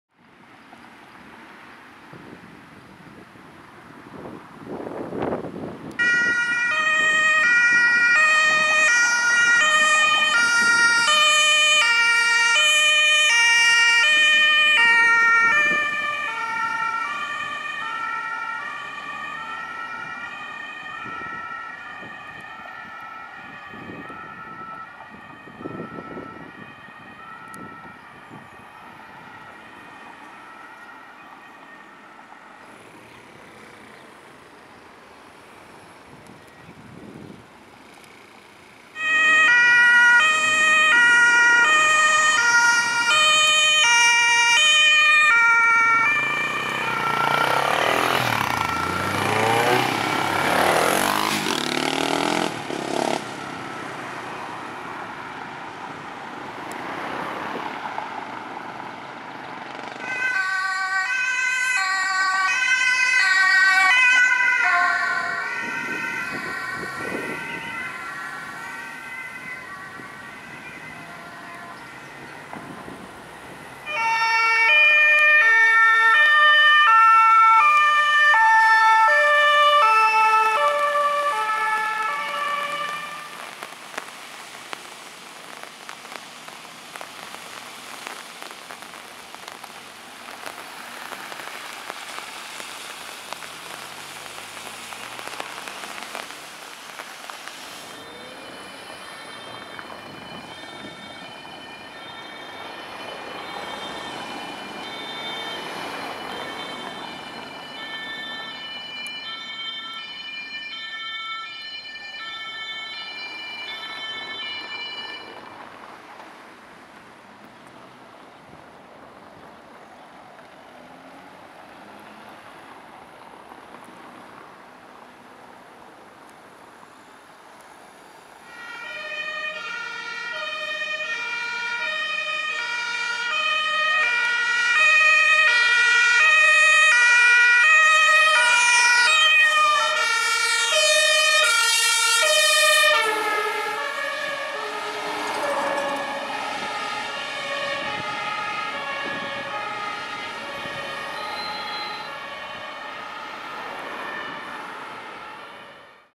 [Vom extremen Regen zum Einsatzabbruch] - Wochenzusammenschnitt (Feuerwehr, Rettungsdienst, Polizei)
Der heutige Wochenzusammenschnitt ist in der Woche vom 5.-11. Juni 2017 entstanden. In dieser Woche fand wieder das Hafenfest in Wismar statt, welches ein Grund für den Zusammenschnitt war.

Ich habe viel verpasst, aber auch verhältnismäßig viel gefilmt. Bei jedem Wetter habe ich mich mehrere Stunden jeden Tag draußen hingestellt. Die Aufnahmen aus dieser Woche seht ihr in diesem Video.

Folgende Fahrzeuge sind in diesem Video enthalten:
(alle Fahrzeuge wurden in Wismar gefilmt und sind auch aus Wismar)

0:00 - RTW DRK
0:32 - FuStW Polizei an Einsatzstelle
0:39 - RTW DRK
1:00 - NEF RW Süd
1:14 - RTW DRK
1:38 - FuStW Polizei mit augenscheinlichem Einsatzabbruch
2:08 - Kurzzug BF

Aufnahmeorte: Bürgermeister-Haupt-Straße, Lübsche Straße, Philipp-Müller-Straße

Anmerkung: Mir ist bewusst, dass auf Mobilgeräten das Thumbnail falsch dargestellt wird. Ich habe versucht, dies zu beheben, habe es aber nicht hinbekommen.